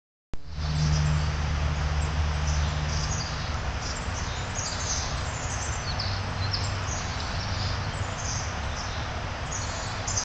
2063 Pool cleaner in operation.
The pool is in the final stages.  The automatic pool cleaner is operational.